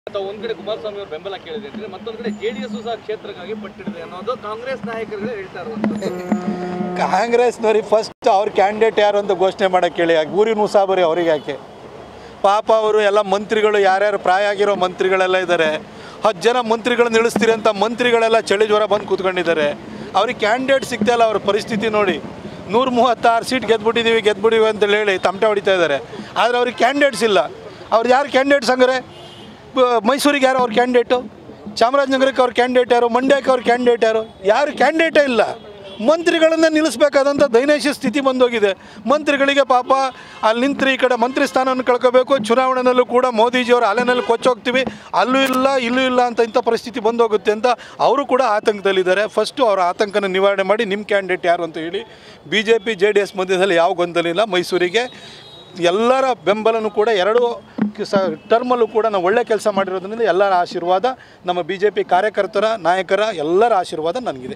ಮಾಜಿ ಸಿಎಂ ಎಚ್.ಡಿ. ಕುಮಾರಸ್ವಾಮಿ ಭೇಟಿ ಬಗ್ಗೆ ಪ್ರತಾಪ್​ ಸಿಂಹ ಫಸ್ಟ್ ರಿಯಾಕ್ಷನ್.!
Btv News Kannada : ಮಾಜಿ ಸಿಎಂ ಎಚ್.ಡಿ. ಕುಮಾರಸ್ವಾಮಿ ಭೇಟಿ ಬಗ್ಗೆ ಪ್ರತಾಪ್​ ಸಿಂಹ ಫಸ್ಟ್ ರಿಯಾಕ್ಷನ್.!